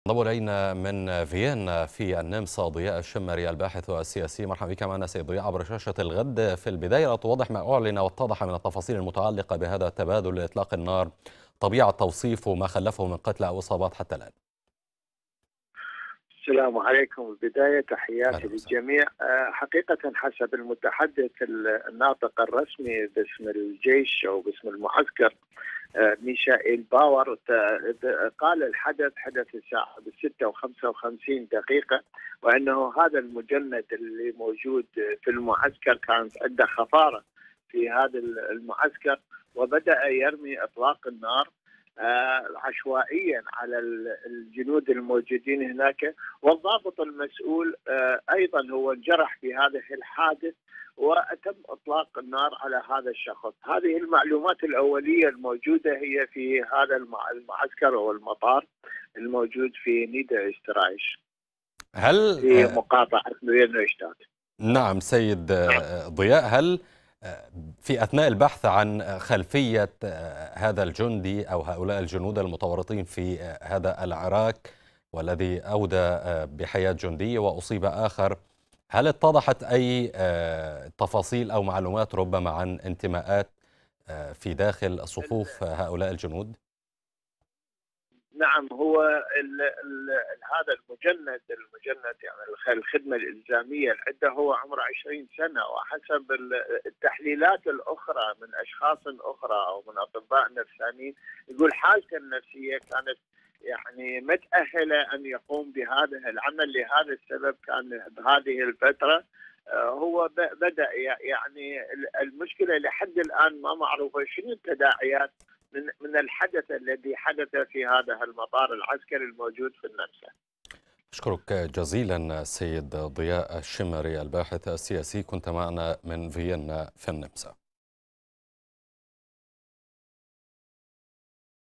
جندي يفتح النار على زملائه في النمسا ويتسبب في قتيل ومصاب
ذكر متحدث باسم وزارة_الدفاع_النمساوية أن جنديا لقي حتفه وأصيب آخر بعد تبادل لإطلاق النار
في ثكنات قاعدة جوية ببلدة فينرنويشتات شمالي النمسا. وأفادت وكالة الأنباء النمساوية بأن جنديا فتح النار أثناء جدال خلال نوبة حراسته، فيما أطلق جندي أعلى رتبة الرصاص على الأول فأرداه قتيلا.
مزيد من التفاصيل مع ضياء الشمري الباحث السياسي في فيينا مع الإعلامي محمود عبد الصمد.